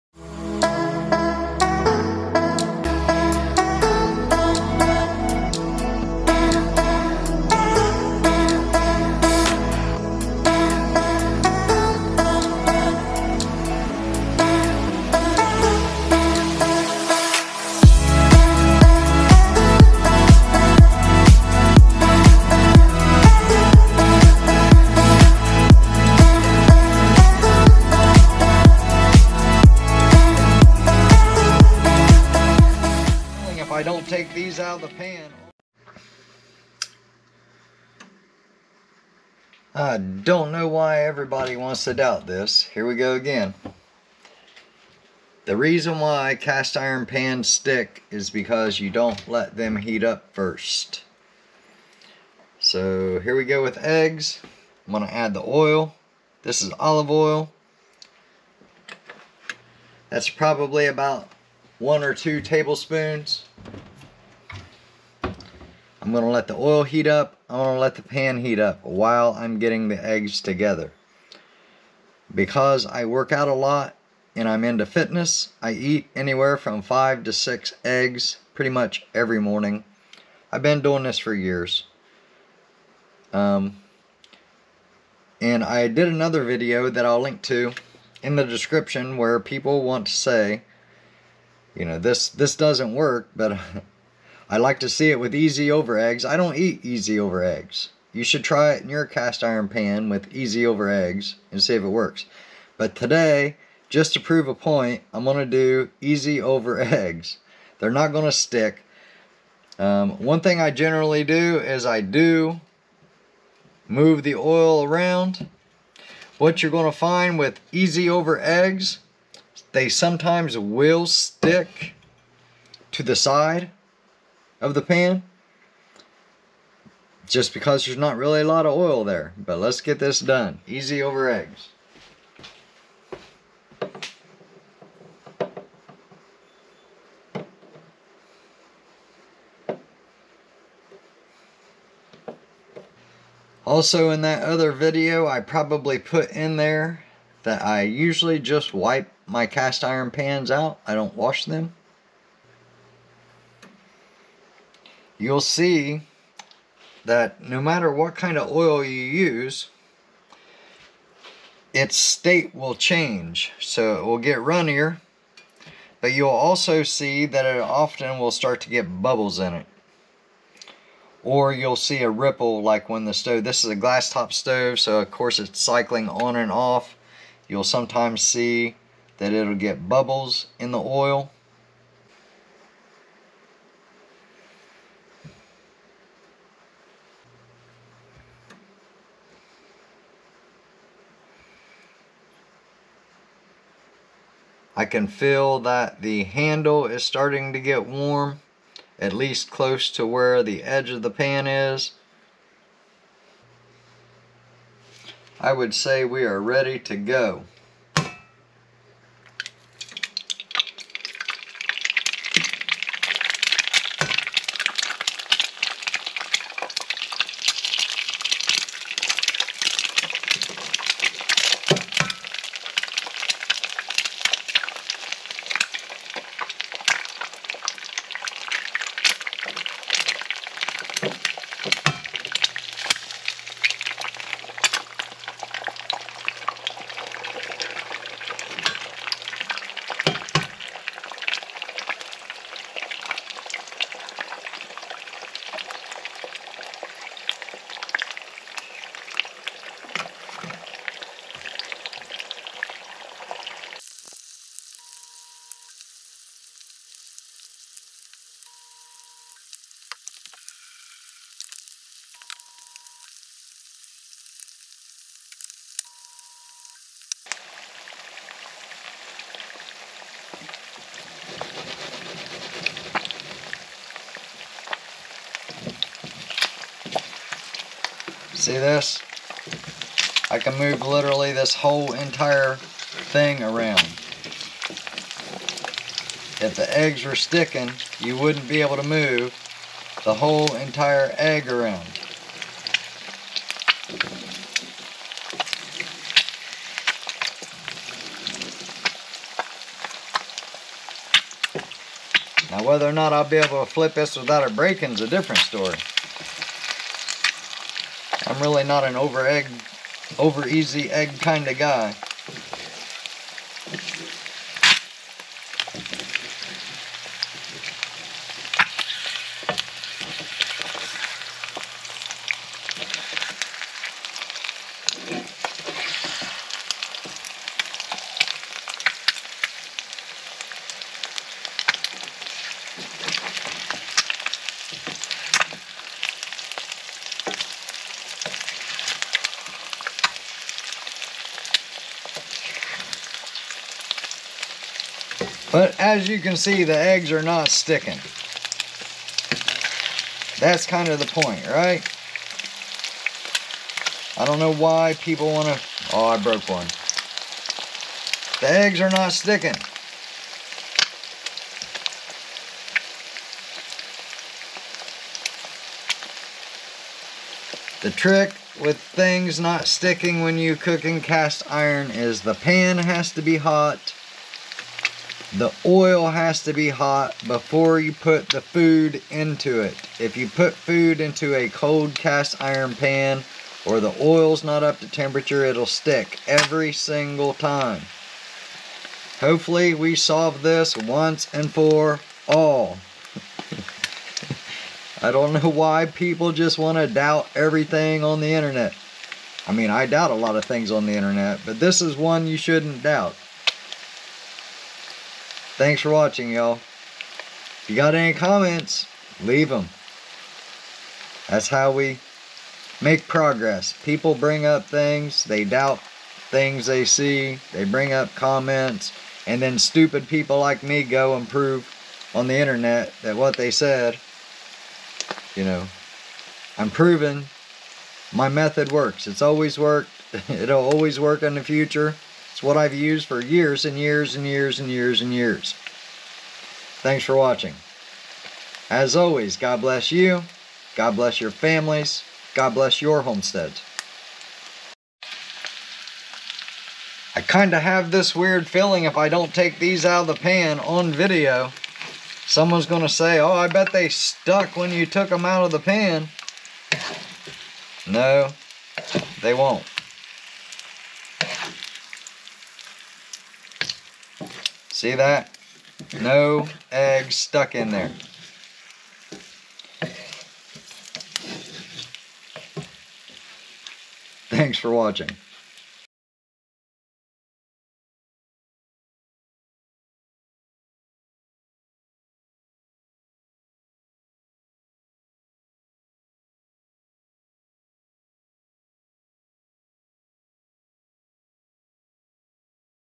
no sticking over easy eggs cast iron pan once & for all
I did a video a couple years ago about cooking eggs in cast iron.  When I got cancer, my future was uncertain and I was going to just "brain dump" as much information as I could to help others.  Well, I beat cancer and the people I tried to help just question everything and doubt my method.  Its annoying!  Why do people constantly have to dispute everything?  I've used this method for years and years and years.  I only own cast iron pans, you would think because of that and because I cook 3 meals a day on them I would know how to use them.

I know someone is going to question the amount of extra virgin olive oil so let me explain that - extra virgin olive oil is one of the most caloric dense foods you can eat, 1 tablespoon is about 120 calories of healthy fats. When I do long distance hiking, I take a bottle of olive oil with me. I drink it like "shots" for quick calorie replacement. Since I eat 5-6 eggs every morning (for 13 years now) for the protein content, I use more olive oil than required for the calories to balance it out. But the process in this video works if you use less oil. If you are a fitness freak, which is why I do homesteading to begin with, then this makes sense. Don't question the amount of oil or my blood pressure or cholesterol, because its posted on my other social media sites ... my blood pressure runs consistently around 103/60, a resting heart rate of 38 - 41, no high cholesterol, and my v02 max is usually 51 - 55. not bad for a 969 year old vampire. Don't make me bite you!